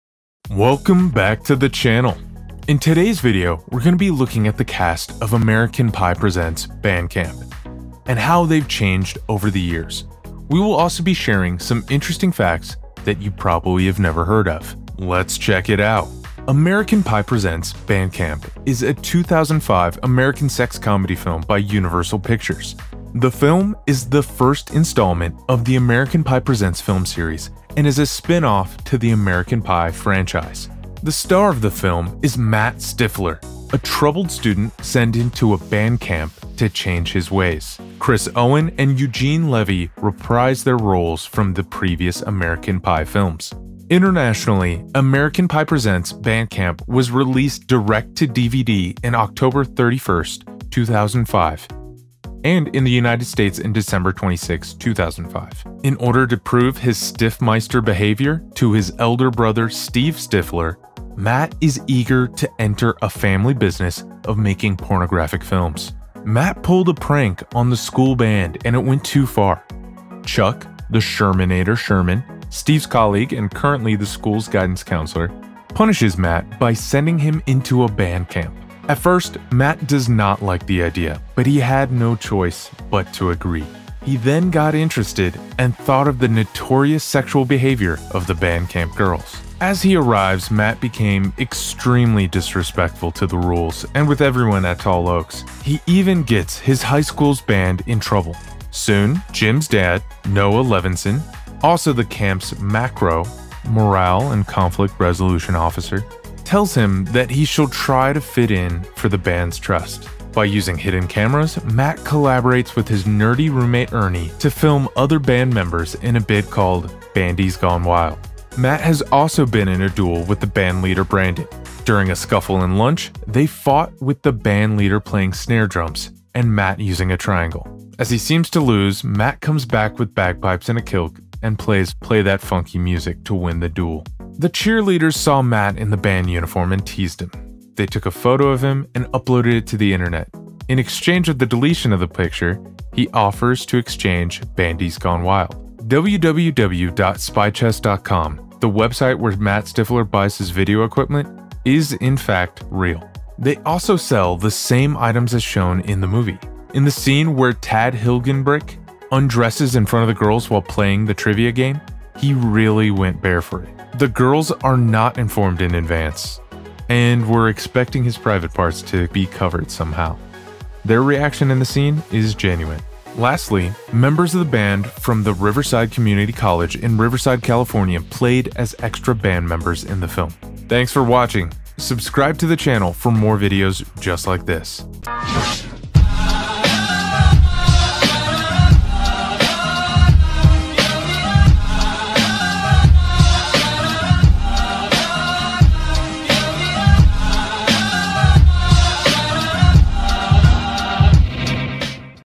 American Pie Band Camp Cast Then and Now 2021
We will be looking at the cast of American Pie Band Camp and how they have changed over the years!

American Pie Presents: Band Camp is a 2005 American comedy film released by Universal Pictures. It is the first installment in American Pie Presents film series, a spin-off of the American Pie franchise.

Main Cast:
Tad Hilgenbrinck as Matthew "Matt" Stifler
Arielle Kebbel as Elyse Houston
Eugene Levy as Noah Levenstein
Chris Owen as Chuck Sherman
Matt Barr as Brandon Vandecamp
Jun Hee Lee as James Chong
Jason Earles as Ernie Kaplowitz
Crystle Lightning as Chloe
Omar Benson Miller as Oscar
Lauren Mayhew as Arianna
Angela Little as Sheree
Rachel Veltri as Dani
Dossett March as Andy
Lily Mariye as Dr. Susan Choi
Ginger Lynn as Nurse Sanders
Richard Keith as Trading Card Bandie
Jennifer Walcott as Bathroom Girl